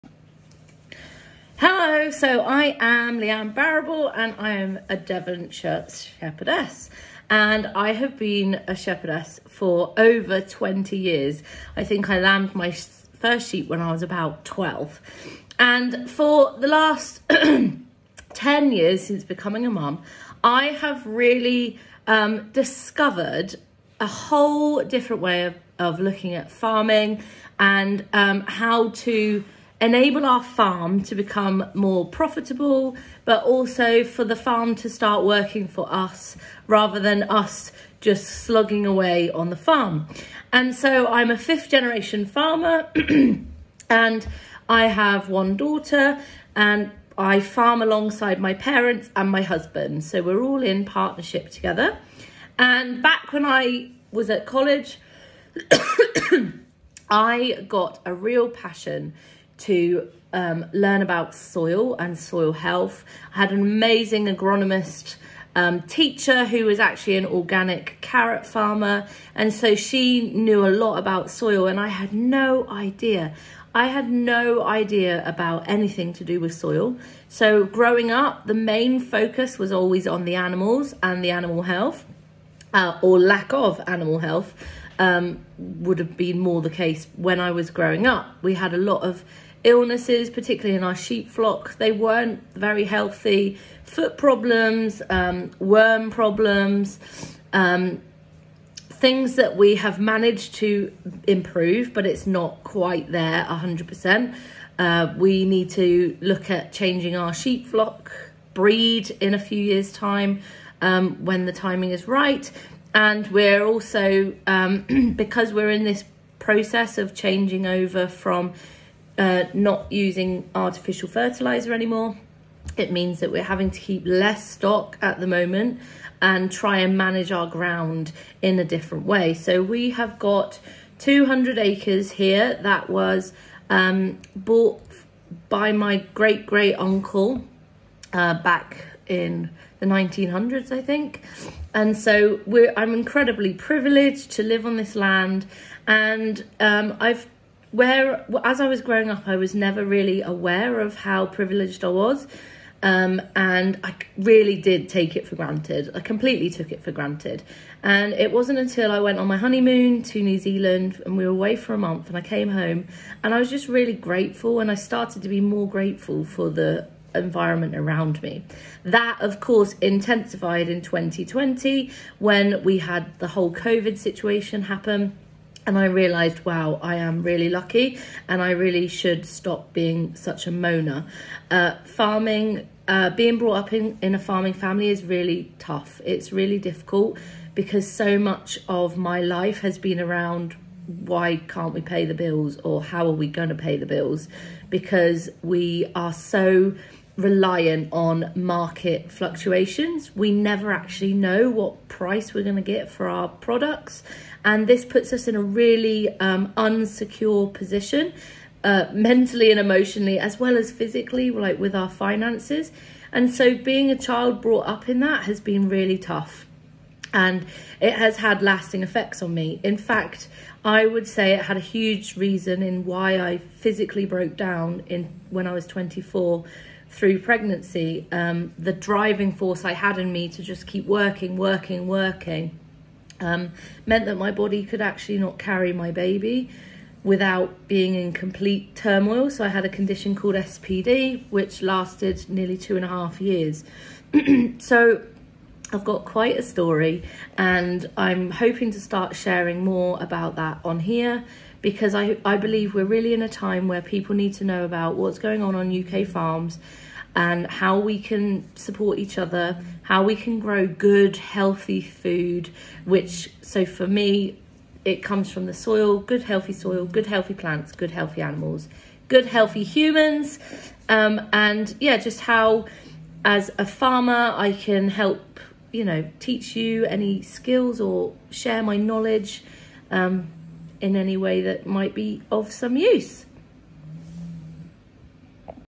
Introducing Leanne Barriball
A little about who I am where our farm is, what we farm and why I started the Devonshire Shepherdess.